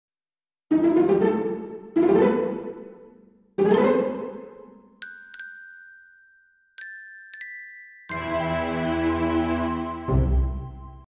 Quail Productions NEW Title Sequence!
A new title sequence that'll appear in my videos from now on. Animated entirely by hand in Adobe Animate.